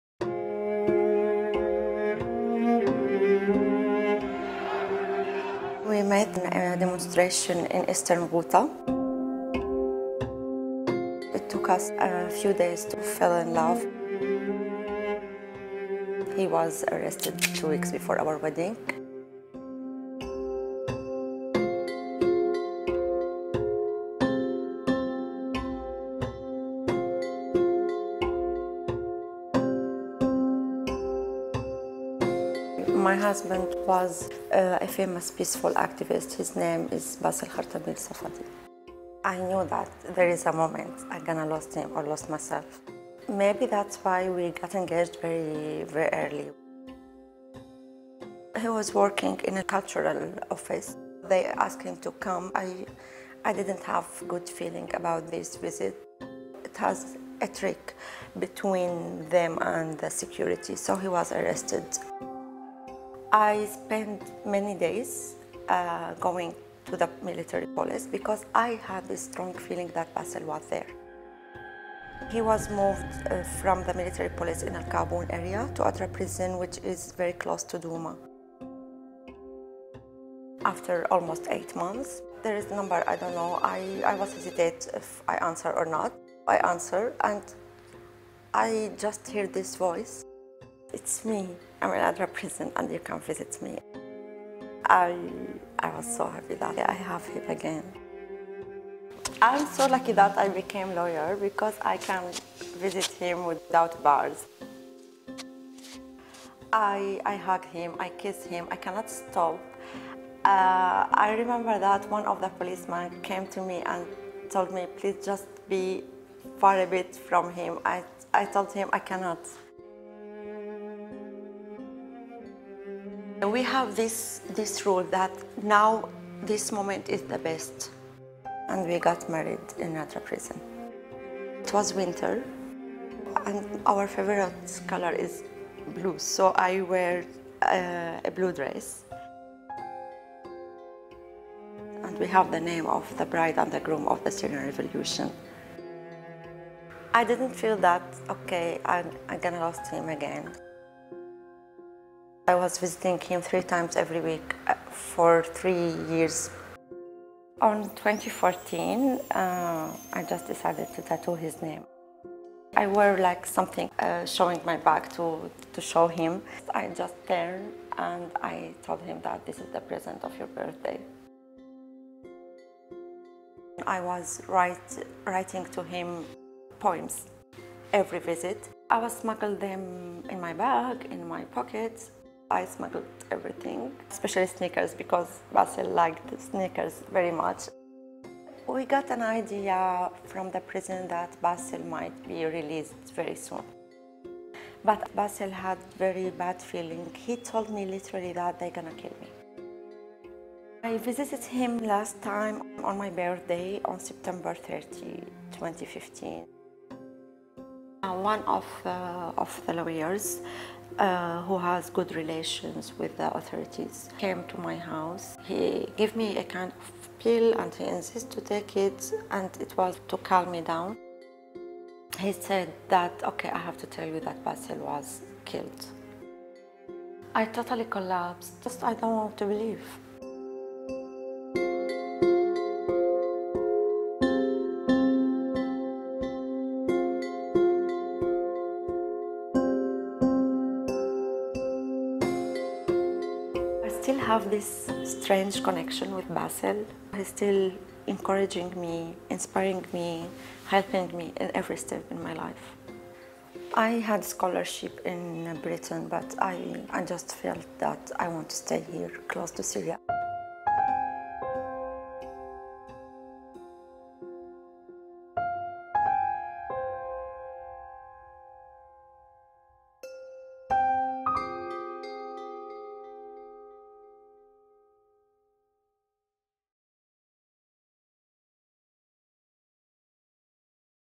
نورا وباسل...عروس وعريس سجن درعا
اعتقلت السلطات في سوريا عشرات الآلاف من السوريين خلال الصراع، وكثير من العائلات لا يعرفون عن أبنائهم شيئا.

أحد أولئك كان باسل خرطبيل صفدي، وكان ناشطا مشهورا.

زوجته نورا، المحامية في مجال حقوق الإنسان تحكي قصتهما وكيف تزوجا في السجن

تقرير إلويز ألانا - رسوم جوناثان بلزارد